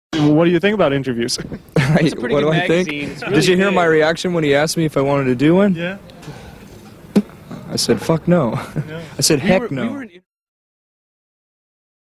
Kurt Cobain: "I Said Fuck No"
What Kurt Cobain really thinks about interviews..